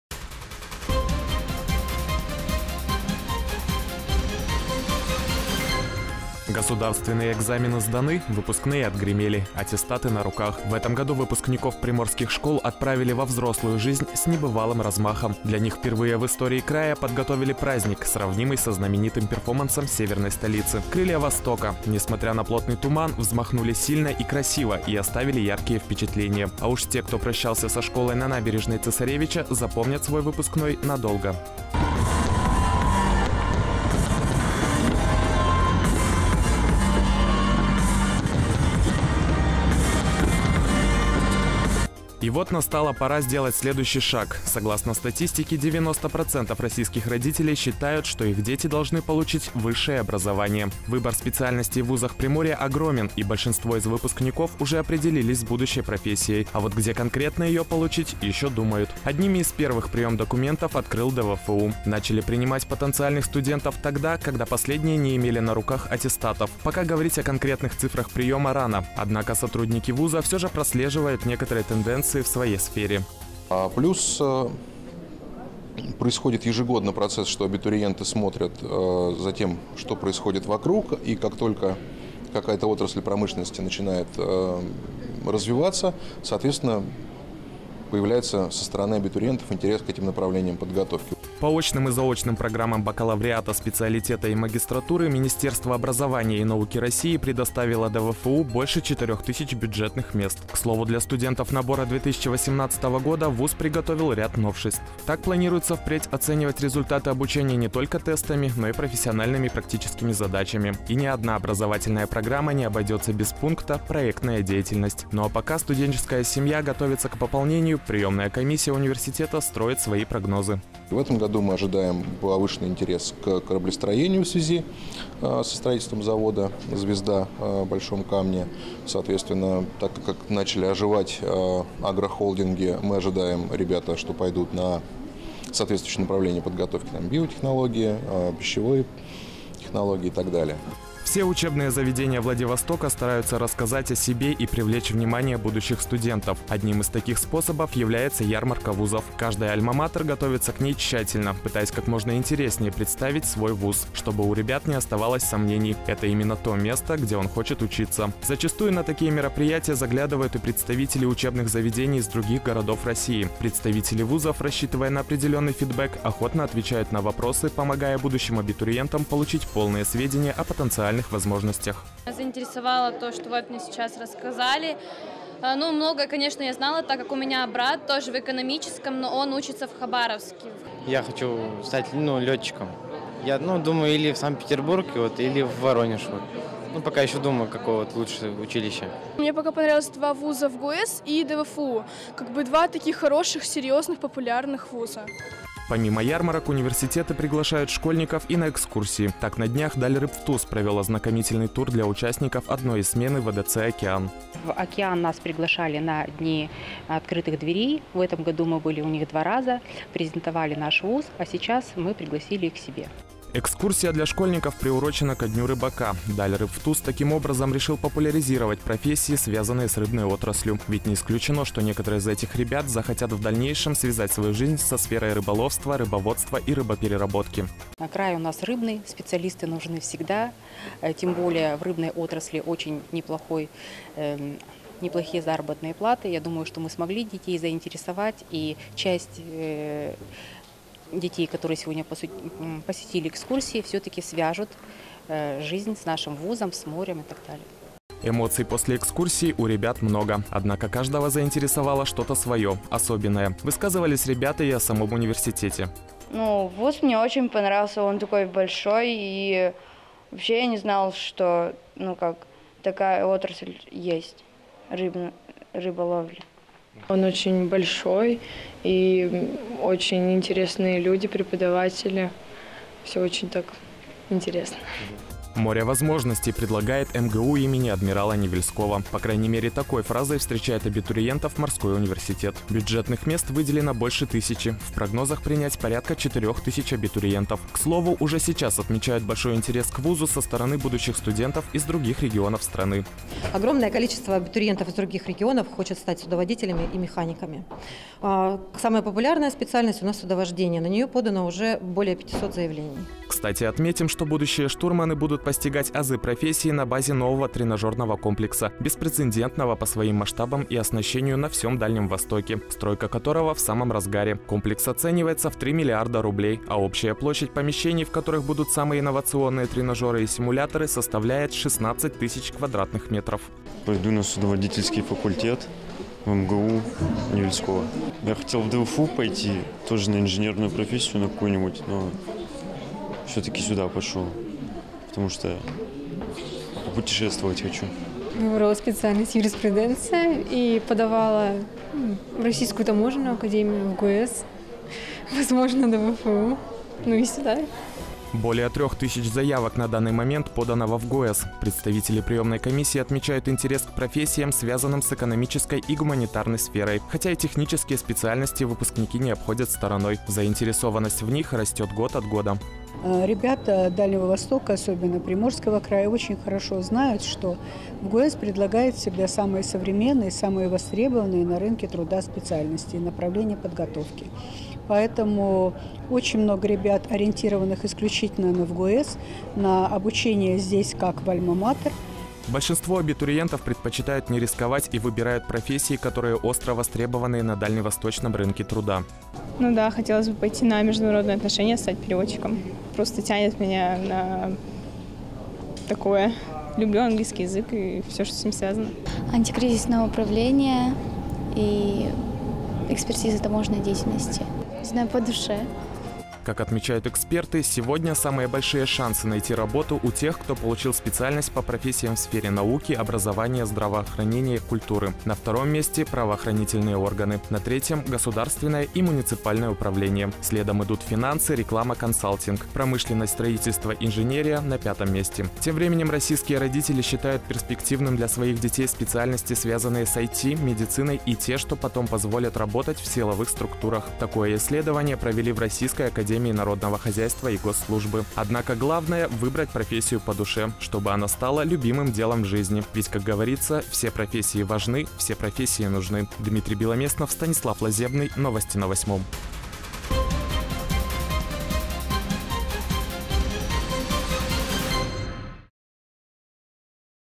Выпускники школ Владивостока выбирают вузы для поступления
В вузах страны, в том числе и во Владивостоке, - пора зачисления абитуриентов по итогам Единого госэкзамена. Как отмечают эксперты, период госэкзаменов прошел в достаточно спокойной обстановке. Именно от результатов ЕГЭ сейчас зависит и будущее молодых приморцев. План приема в вузы по стране в 2018 году расширен на 9 тысяч мест. В общей сложности он превышает 514 тысяч мест. Самое большое число бесплатных мест на бюджете в этом году получат педагогические факультеты и педвузы, IT-направления, сельскохозяйственные и медицинские вузы. А вот меньше всего бюджетных мест предусмотрено для таких специальностей как Сестринское дело, Оружие и системы вооружения, Искусствознание, Теология, Сценические искусства и литературное творчество. У родителей абитуриентов свои  приоритеты.   42% родителей считают  IT-специальности наиболее перспективной сферой. Армию, полицию, силовые структуры — для своих  чад  предпочли  бы  39%, медицину -  38%. Повысился интерес к инженерным специальностям: если в 2016 году их считали желанными для своих детей 20% родителей, то в 2017-м — уже  32%. В то же время, утверждают исследователи, отмечается падение интереса семей к экономике и юриспруденции.